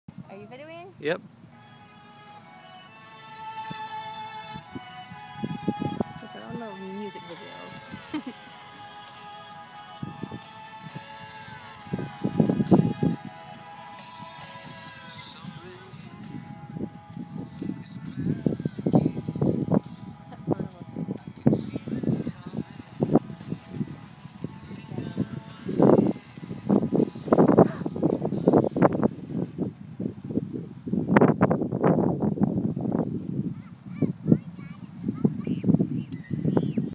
Runaway kite
Isaac let's go of kite and off it goes.